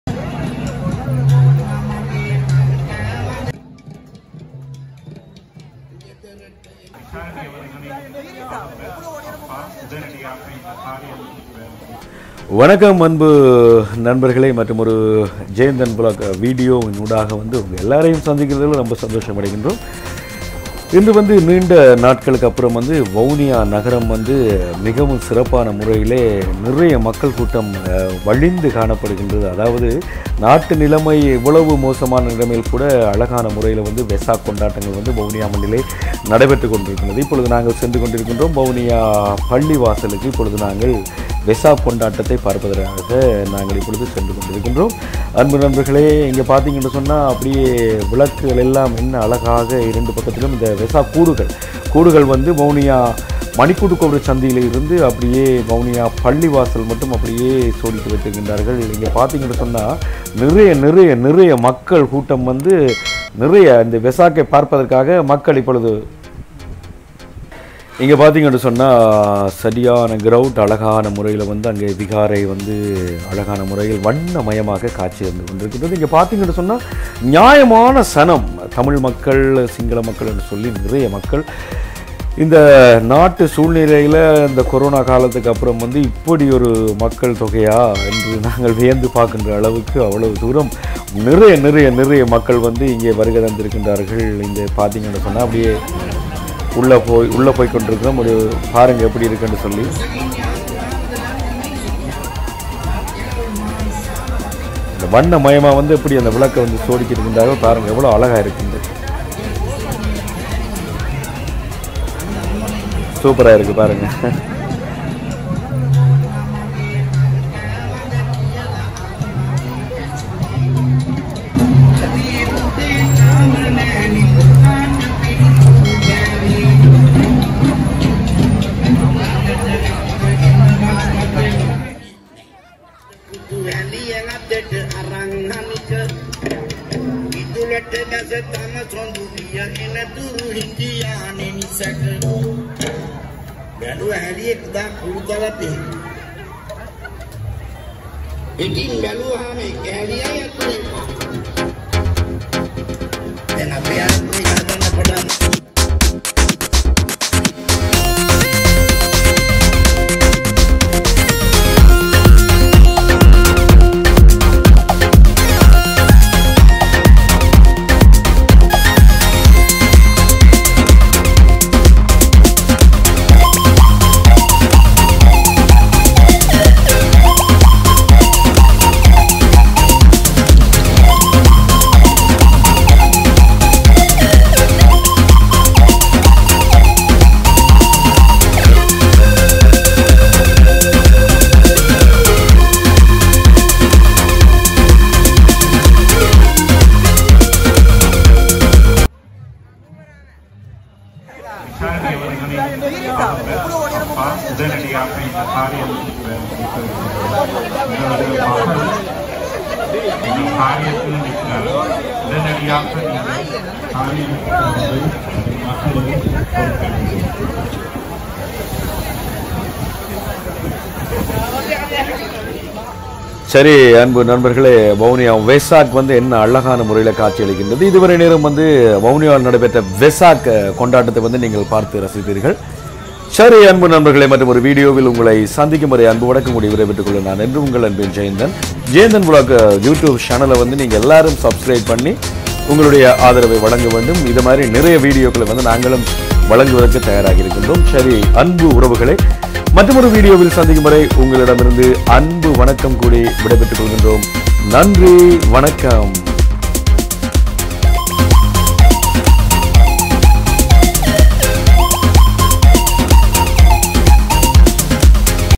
நடைபெற்றுமுடிந்த வவுனியா வெசாக் கொண்டாட்ட நிகழ்வுகள் |VavuniyaVesak 2022 | Jeyanthan Vlogs
vavuniya Vesak 2022
Srilanka Vesak

My social websites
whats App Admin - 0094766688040